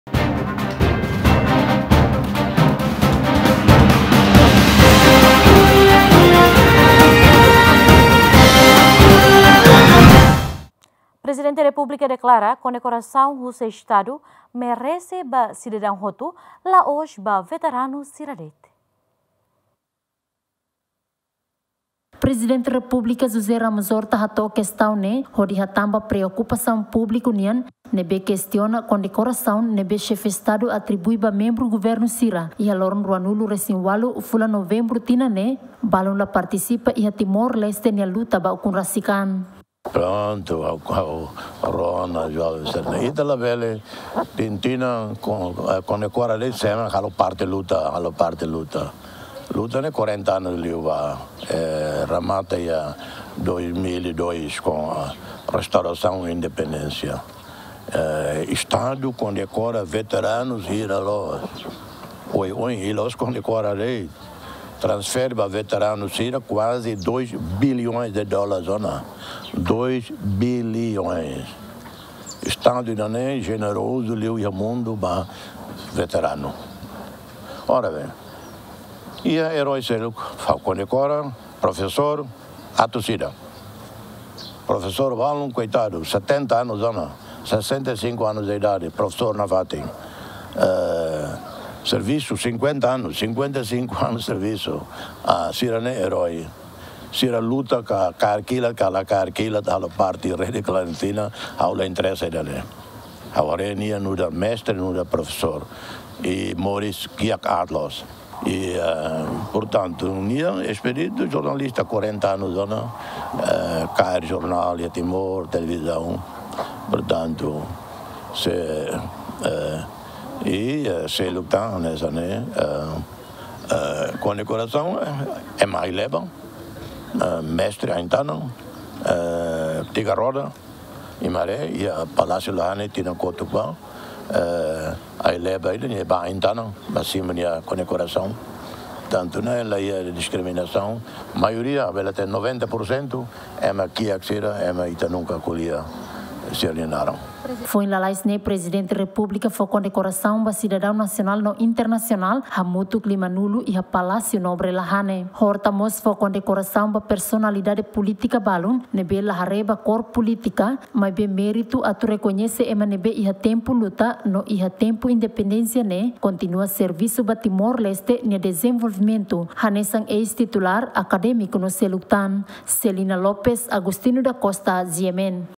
PR Horta deklara kondekorasaun husi Estadu merese ba sidadaun hotu la’os ba veteranu sira de’it
Prezidente Repúblika deklara kondekorasaun husi Estadu merese ba sidadaun hotu la’os ba veteranu sira de’it